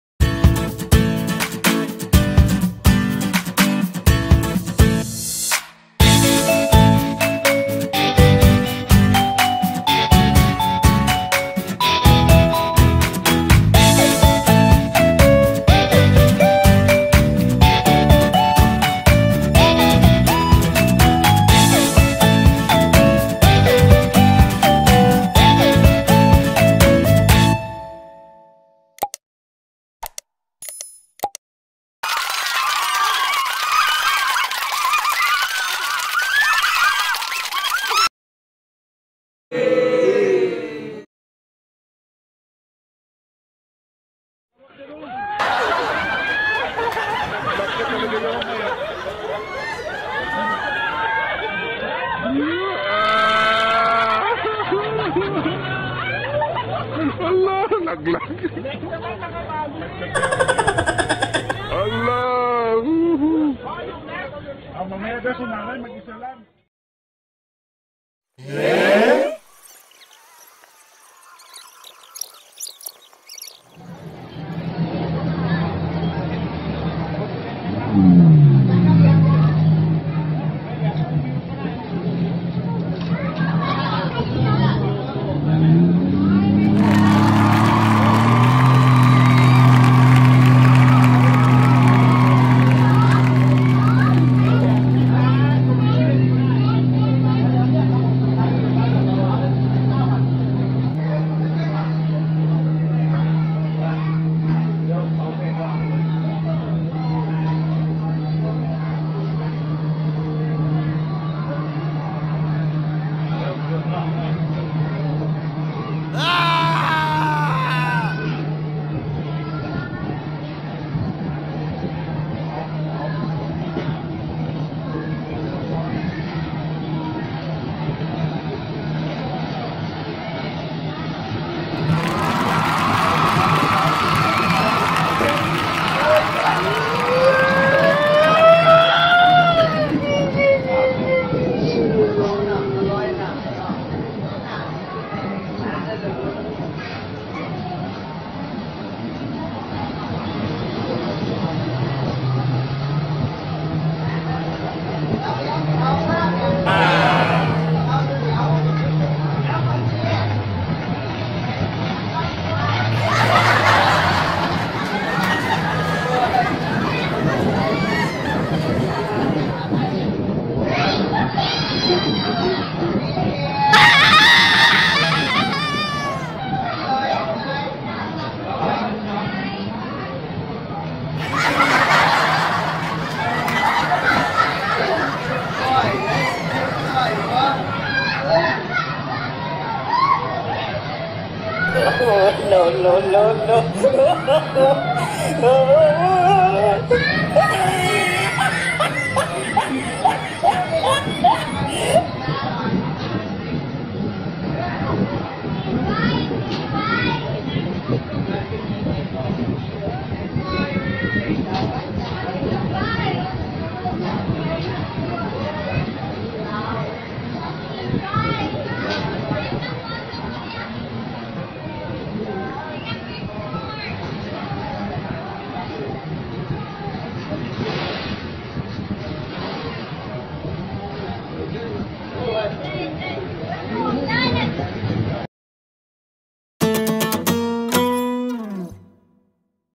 First ride Tubing water at Pattaya Noi Ubon Ratchathani
This is one of the beautiful beach tourist spot of Ubon Ratchathani. First time ko ito guys. it feels so excited, nervous because I'm scared maybe I will fall in the water, but super tired and enjoyable guys.